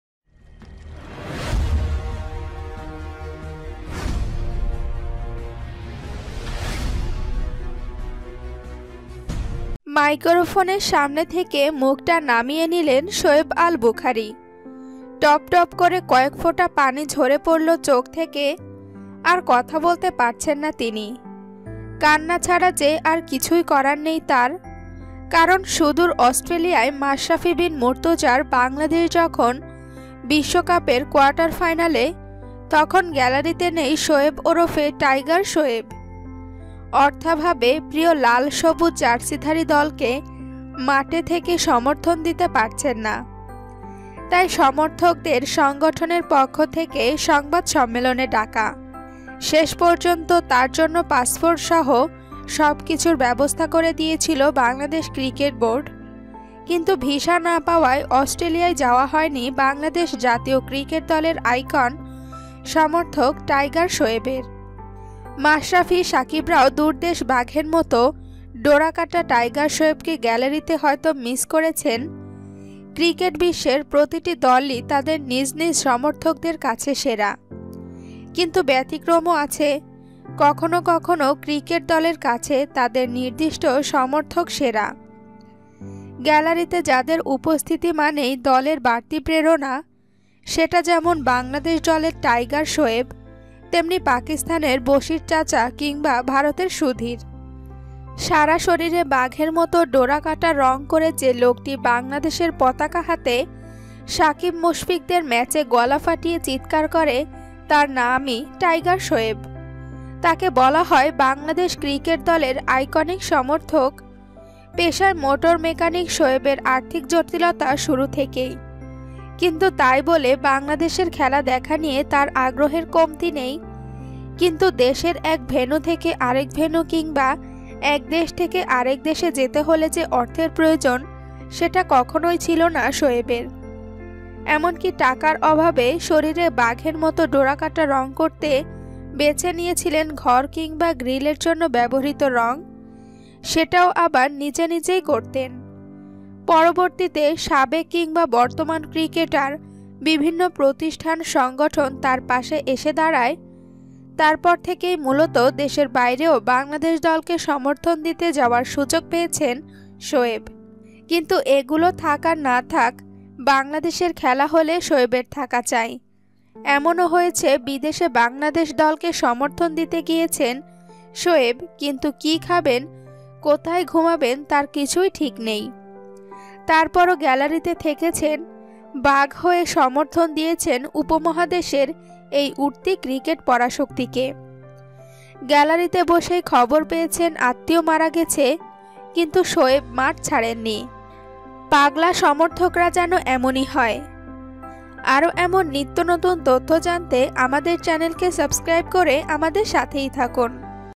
কে এই মোটর মেকানিক শোয়েব আলী? যিনি বাংলাদেশ হারলেই কাঁদে । যার আসল পরিচয় জানলে কেঁদে দিবেন
গ্যালারিতে বসেই খবর পেয়েছেন আত্মীয় মারা গেছে, কিন্তু শোয়েব মাঠ ছাড়েননি। পাগলা সমর্থকরা যেন এমনই হয়।

▶ হযরত আবদুল কাদের জিলানী (রহ.) এর জীবনী